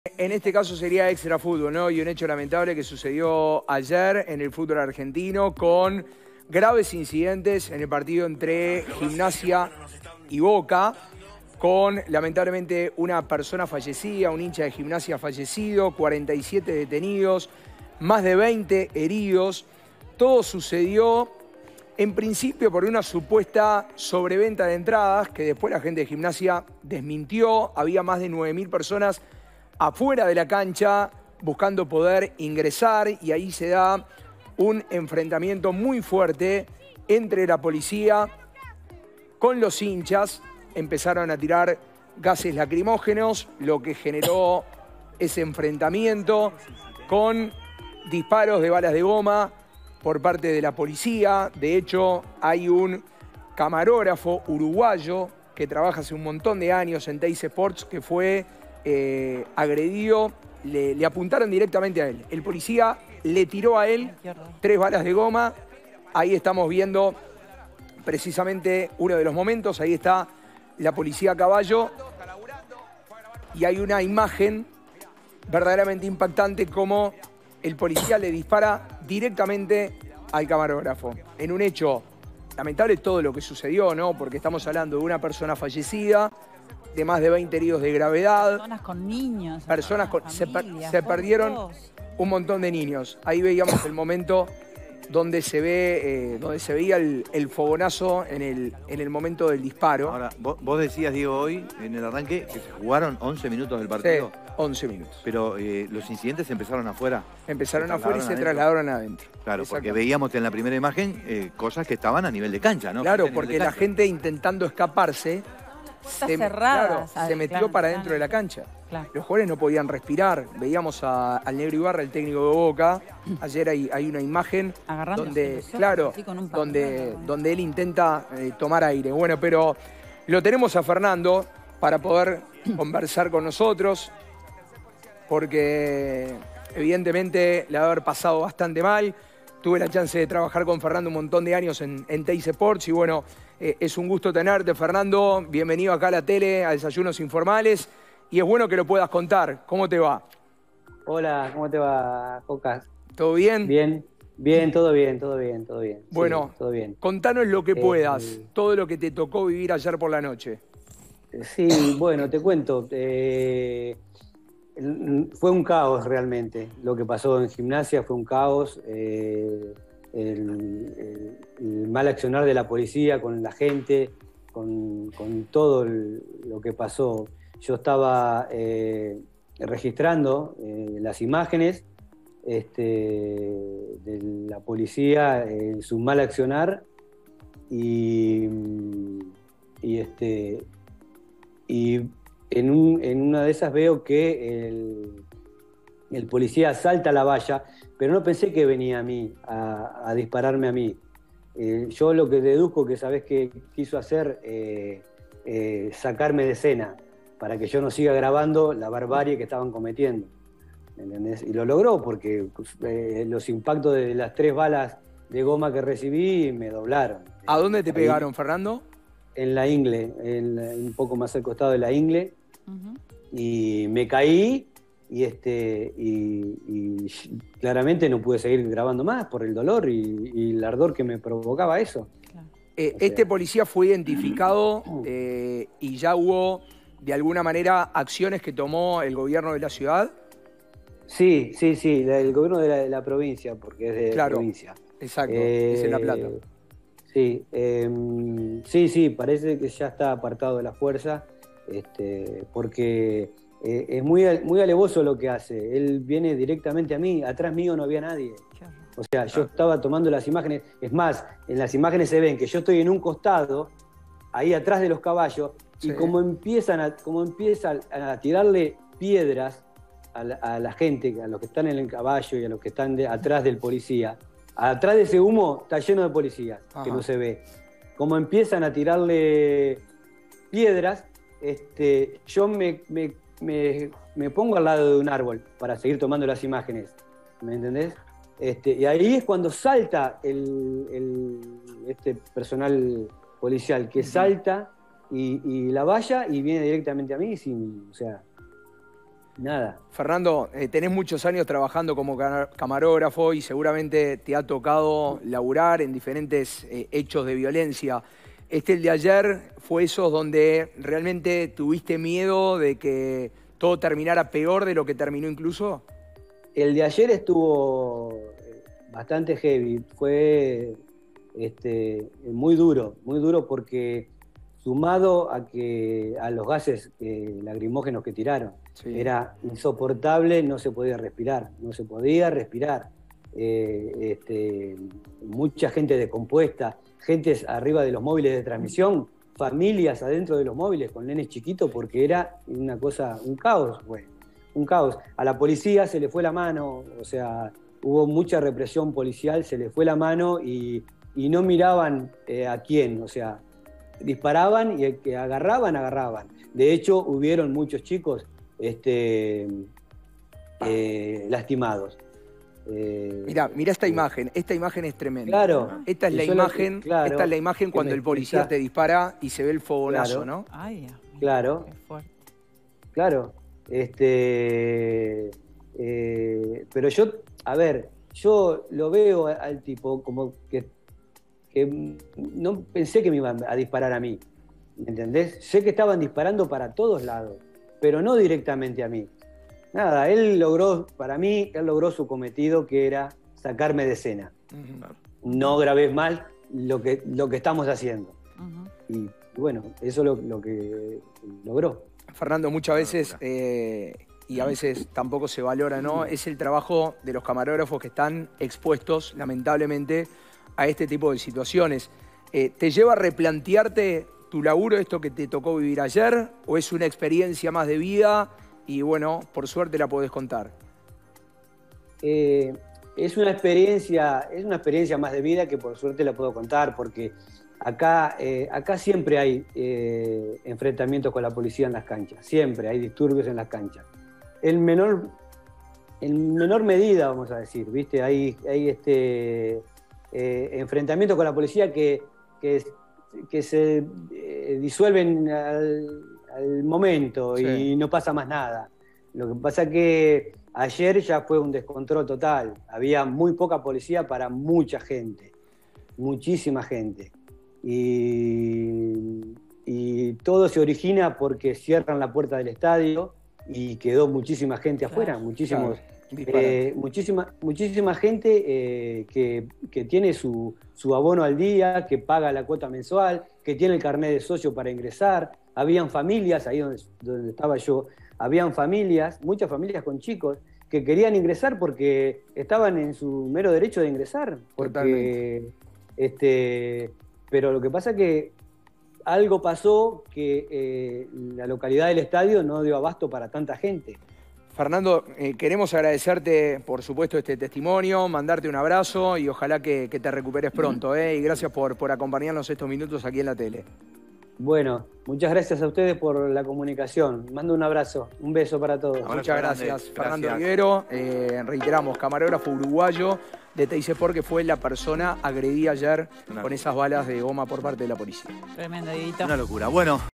El relato de Fernando Rivero: camarógrafo de TyC Sports baleado por un policía
Diego Jokas contactó con el camarógrafo uruguayo que trabaja para el canal deportivo argentino, quien fue baleado por un policía en los graves incidentes durante el partido de Gimnasia y Boca Juniors por la Liga Profesional Argentina, en el cual falleció un hincha de Gimnasia y dejó decenas de heridos.